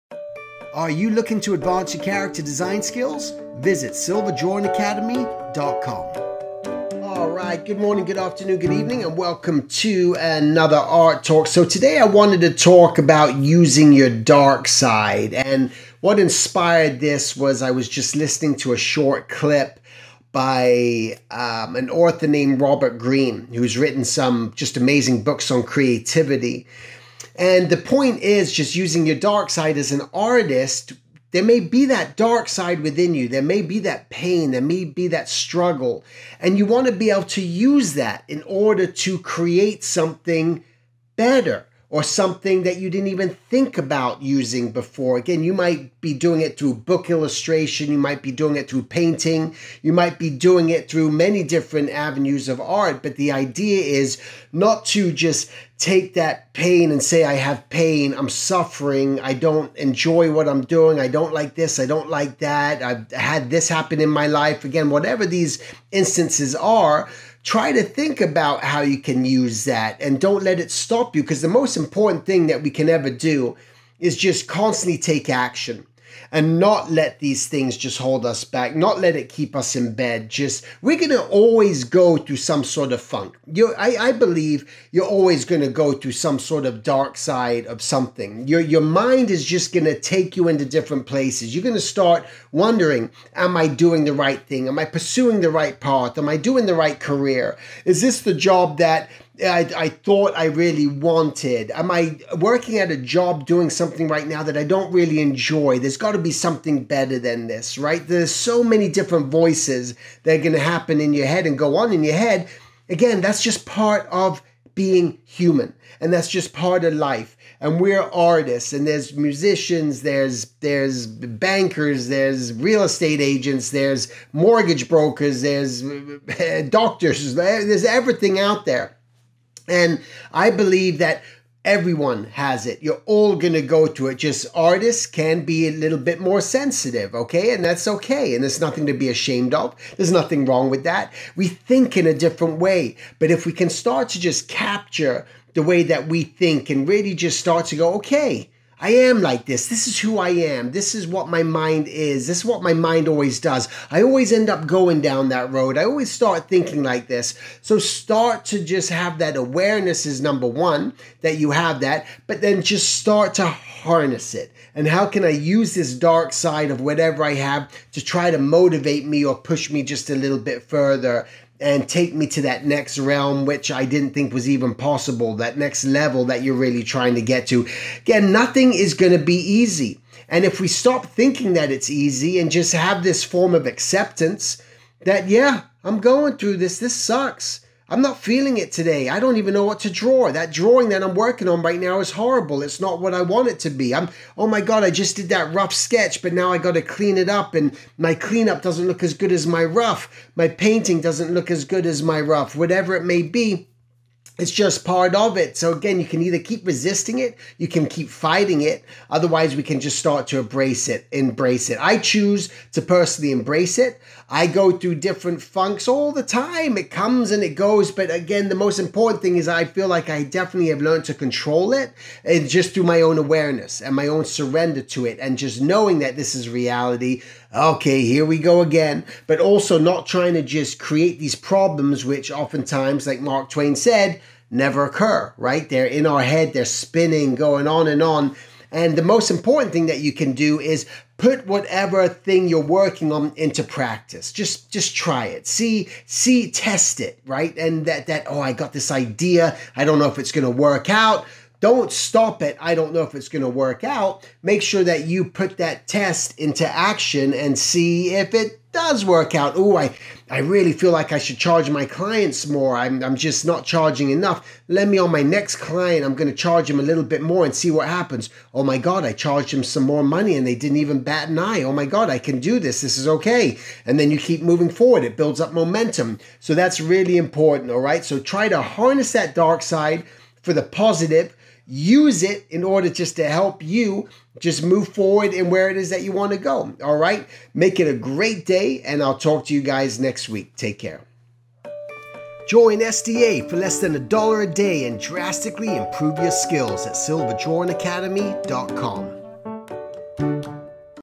Art Talk |Embrace Your Dark-side | Stephen Silver
Stephen Silver; Animation Character Designer of Kim Possible, Danny Phantom, Clerks, and more

Learn about the art of Character Design. Join SDA for less than a dollar a day and drastically improve your skills.